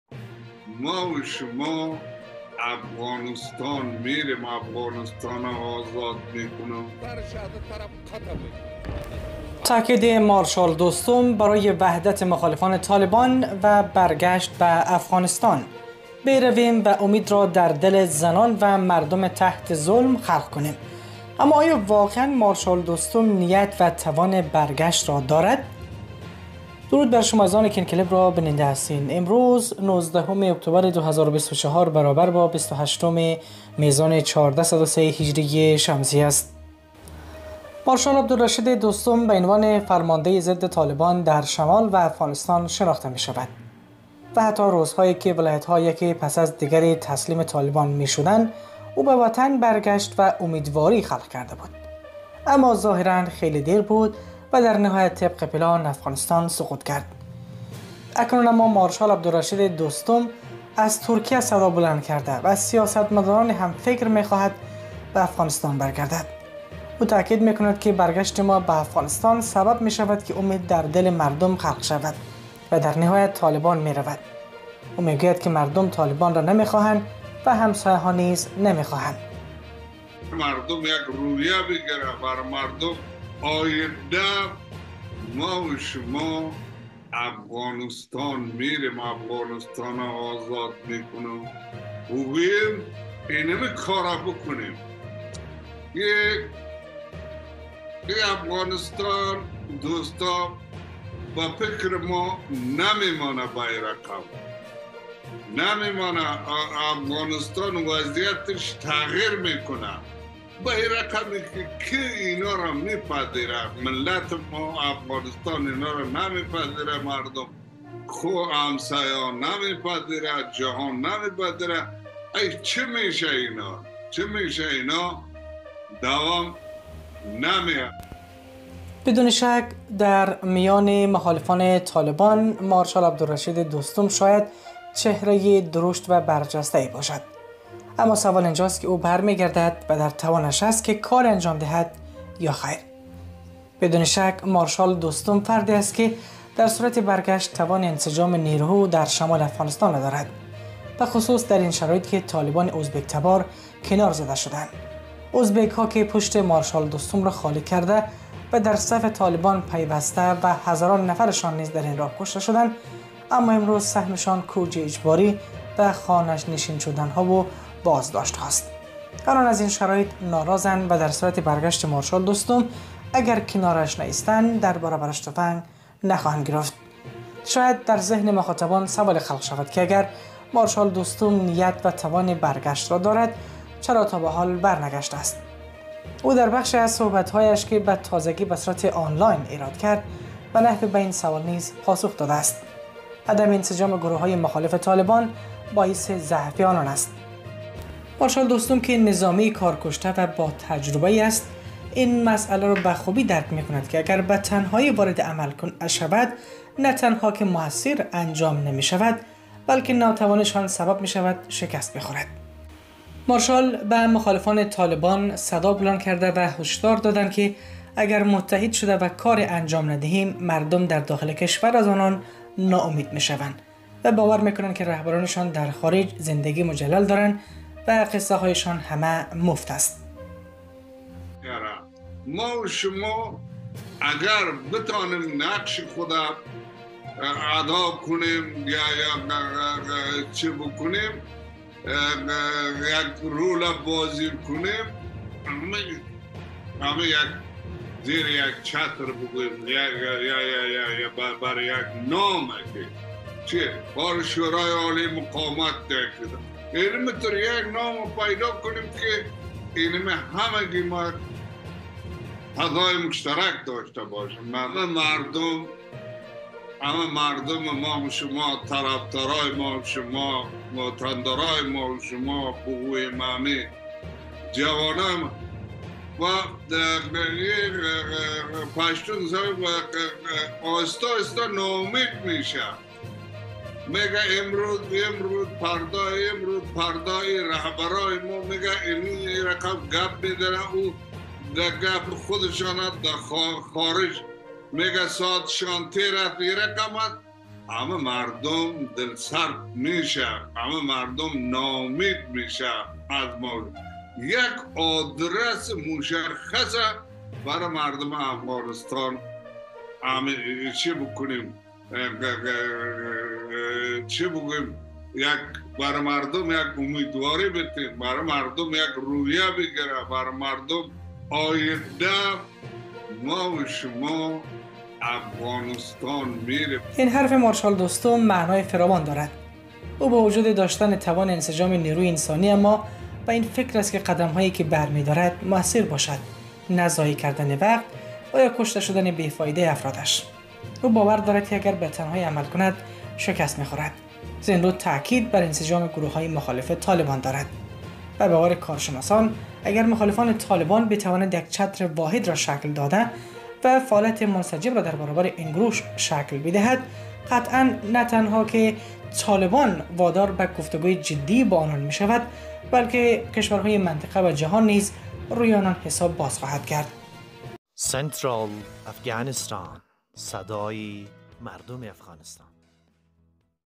مارشال_دوستم برای نبرد سخت با طالبان آماده میشود! | آیا افغانستان بر می گردد و برنامه اش چیست؟
مارشال دوستم برای نبرد سخت با طالبان آماده میشود! | میرویم و امید را در دل مردم زنده میکنیم | | نبرد سنگین در راه است؟
آیا واقعا مارشال دوستم توان و نیت رفتن به افغانستان را دارد؟
بیان این حرف های دوستم قطعا جسارت میخواهد مگر نه؟
برداشت شما چیست؟